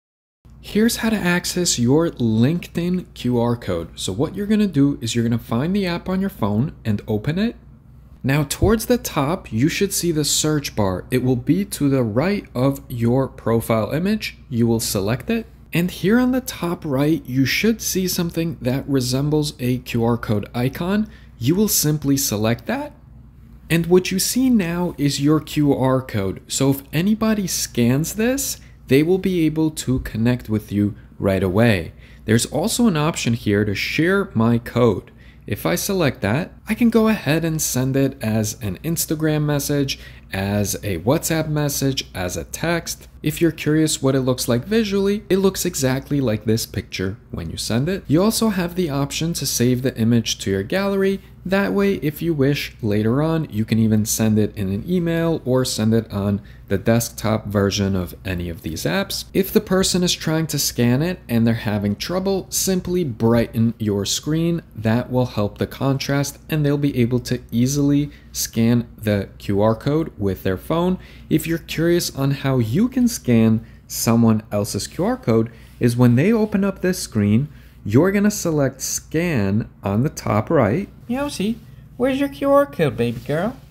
LinkedIn QR Code to Connect Easily (Full Tutorial)
Full Tutorial on how to easily connect on LinkedIn using the QR code feature and where to find it.